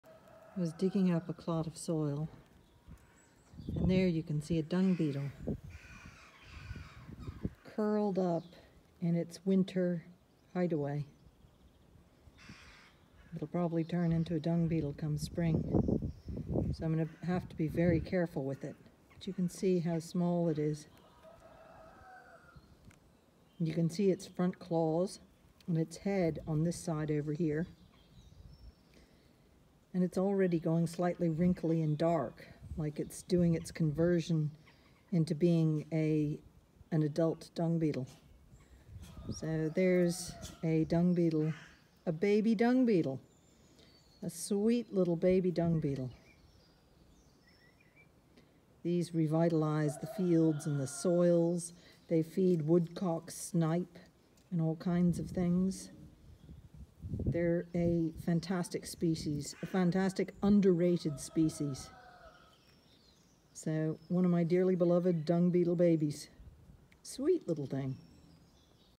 I mistakenly dug up a baby dung beetle, curled up & cozy in soil
This dung beetle larvae curled up in its cozy winter slumber chamber. It means we’ve loads around here on the farm. Dung beetles are becoming rare as modern agriculture practices do not suit this important insect to soil health and biodiversity.